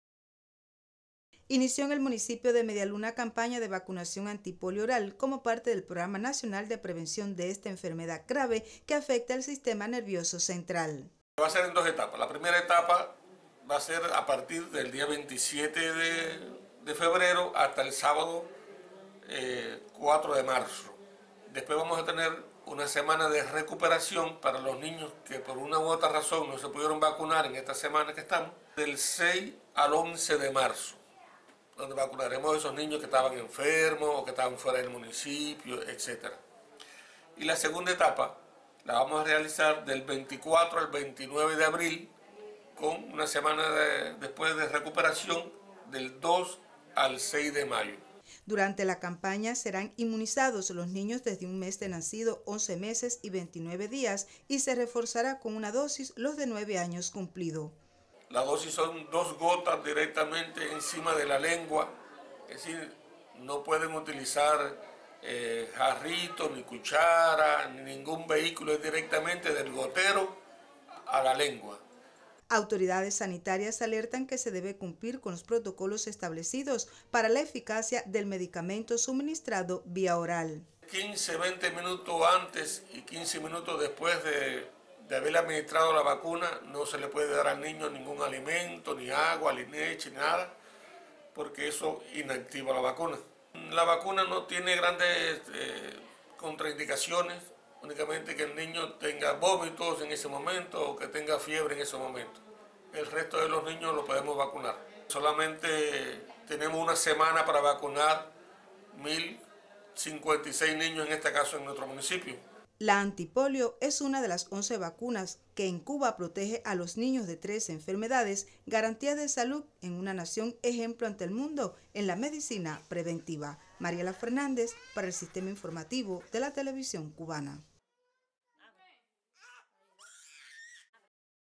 Inicia en Granma campaña de vacunación antipoliomielítica
Serán vacunados todos los niños y niñas que desde el 27 de febrero tengan más de un mes de nacidos, o no hayan cumplido tres años de edad.  En el municipio de  Media Luna más de mil niños serán inmunizados en la presente campaña que se extiende hasta el 29 de abril, con recuperación entre el 2 y el 6 de mayo.